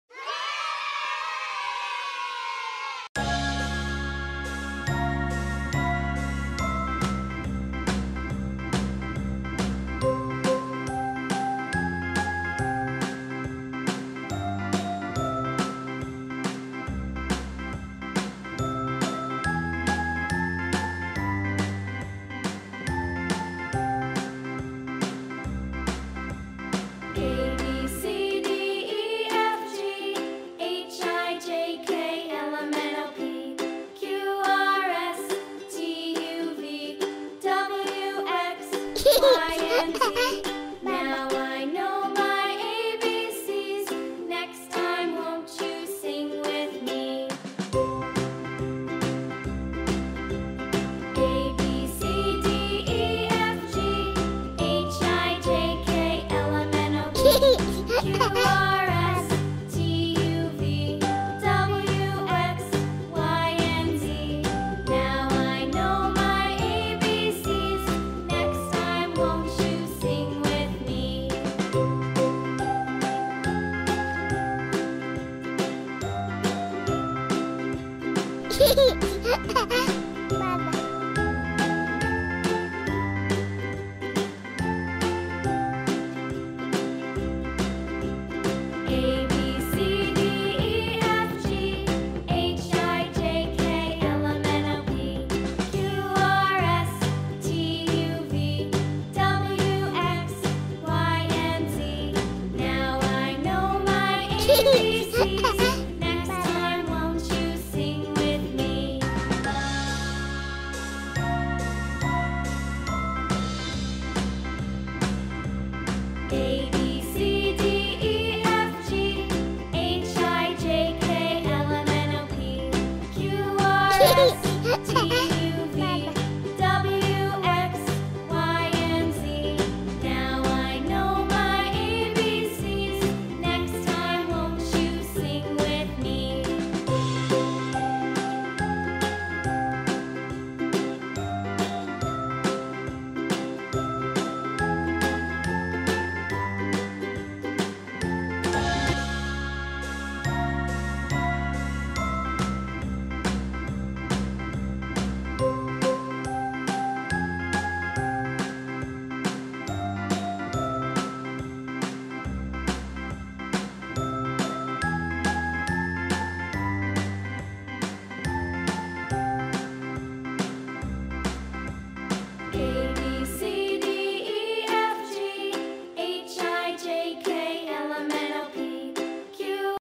Cake Drawing,painting and coloring for kids| Toddler| how to draw
Hello everyone! Today I'm drawing a cake this look colorful, and yummy!I love drawing and painting, please try to draw, paint with m.